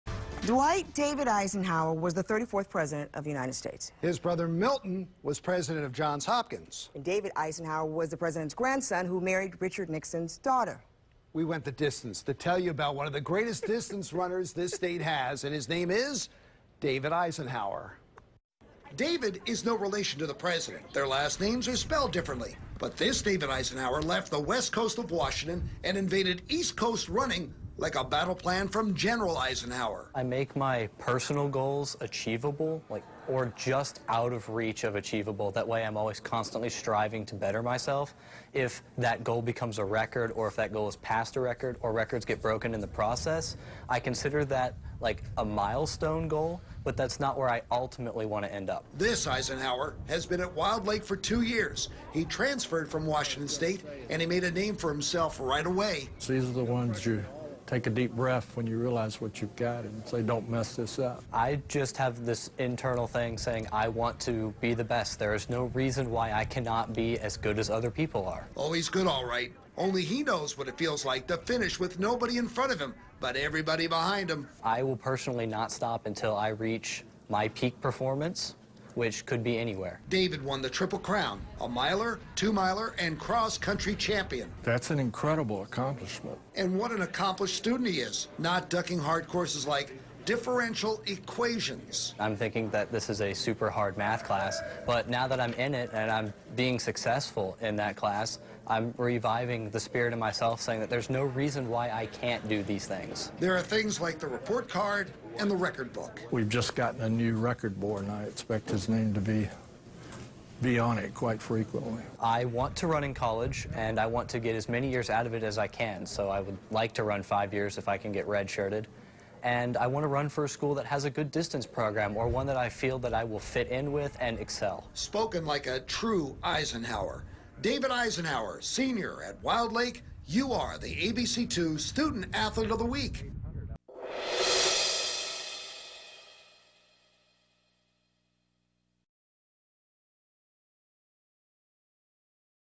Student Athlete of the Week: David Eisenhauer
He runs at the front of the pack, in the classroom and on the track. MORE